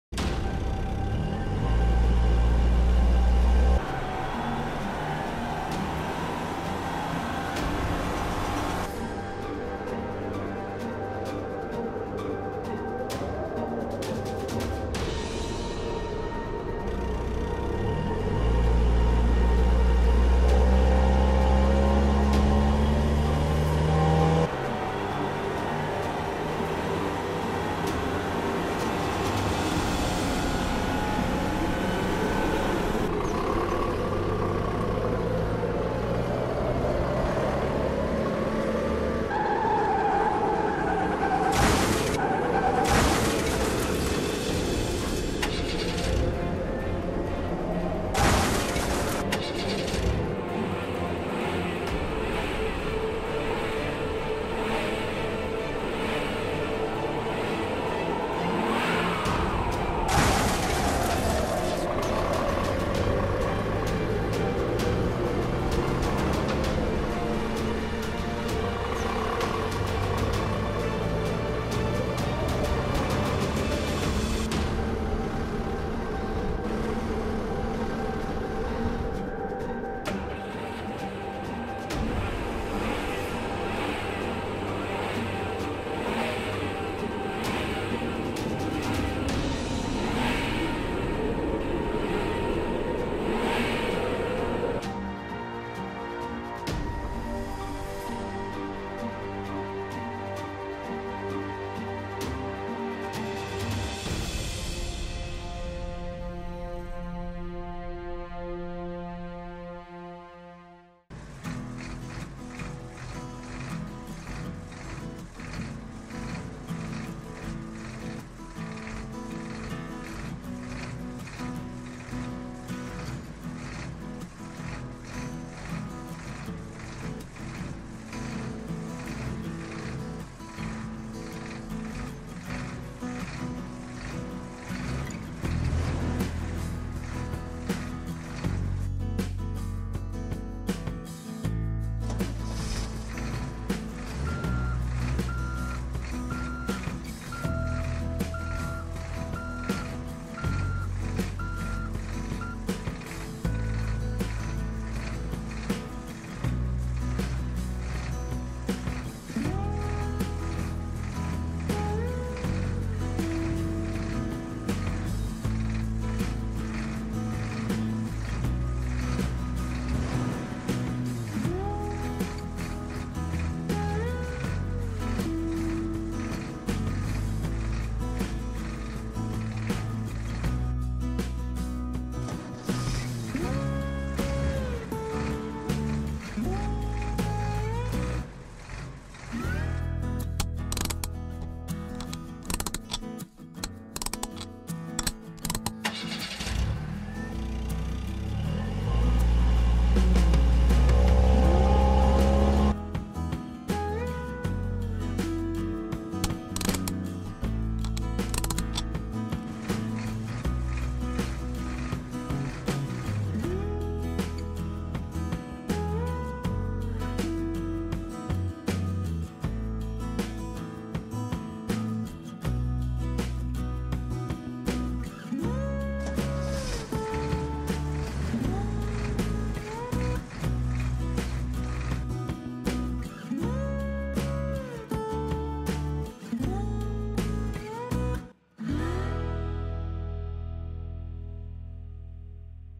Автовоз привез полицейские машины в автосервис на ремонт 441 Серия Мир Машинок
Всем привет, Сегодня в городе прошла очень большая буря. Многие машинки успели выехать за город, в городе осталась только полиция. Сильный ветер перевернул все полицейские машины. После бури на помощь полиции приехал большой автовоз, который и перевез всю полицию в автосервис на ремонт.